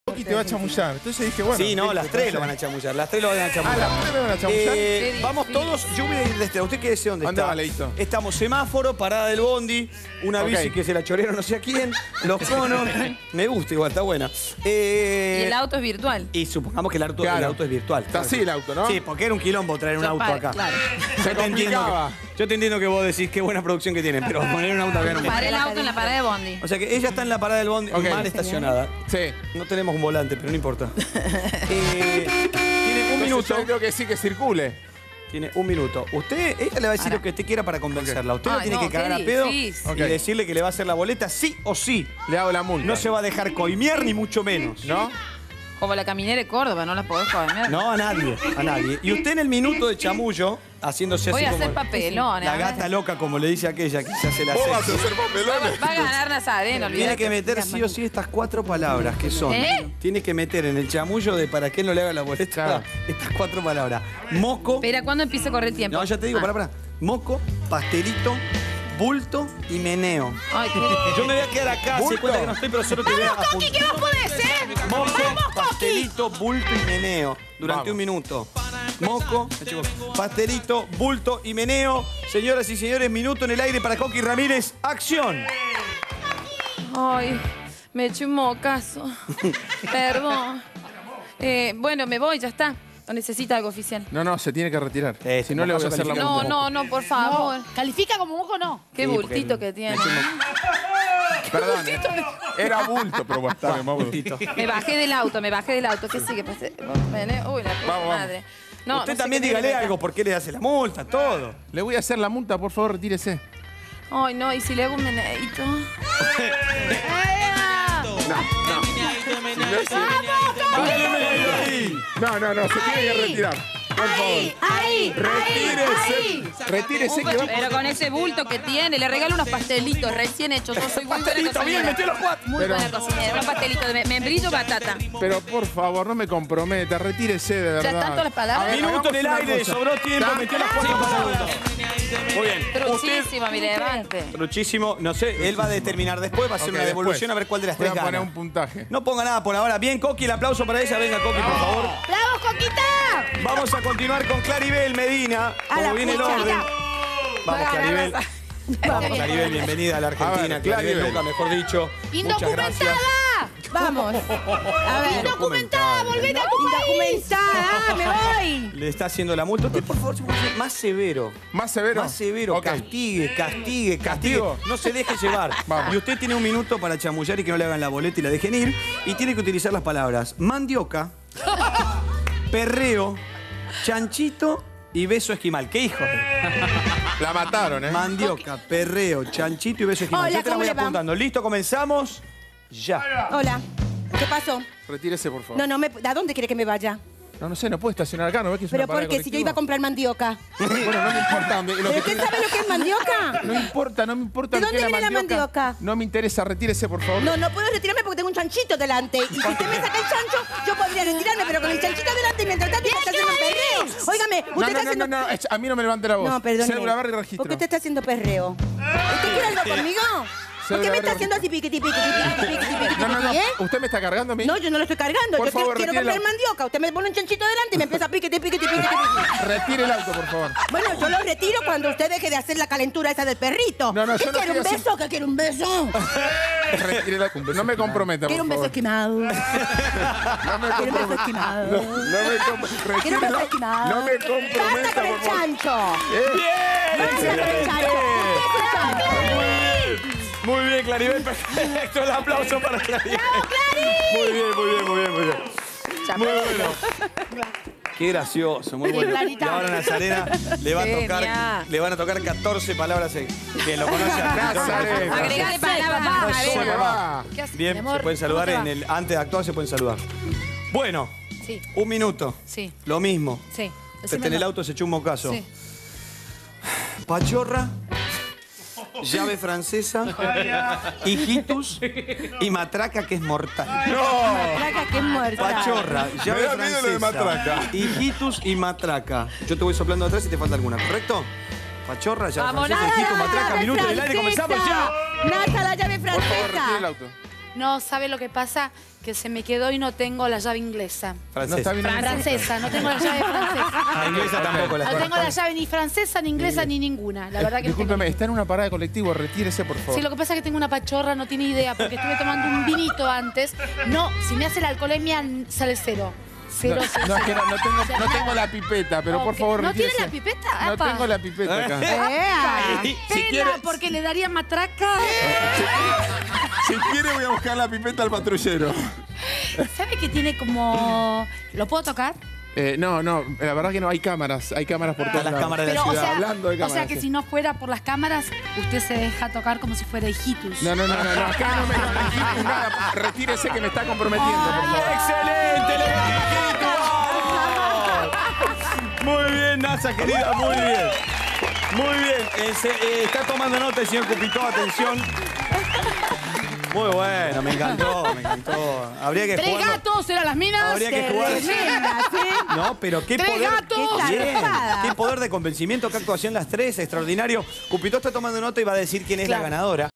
Desafío MDN: El arte del chamuyo
Nuestras bellas y experimentadas invitadas deberán demostrar todo su talento en la seducción, y además, usar cuatro palabras claves que les daremos a continuación ¡Mire!